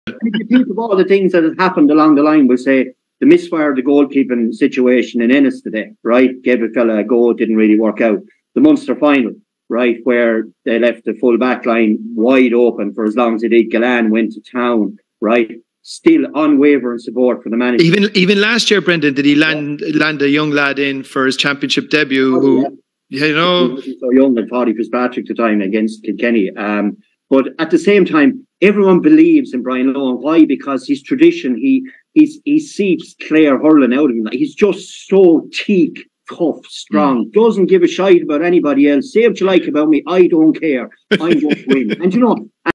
Brendan Cummins Launches Scathing Attack On Brian Lohan
Kilkenny v Clare - 2023 Hurling Championship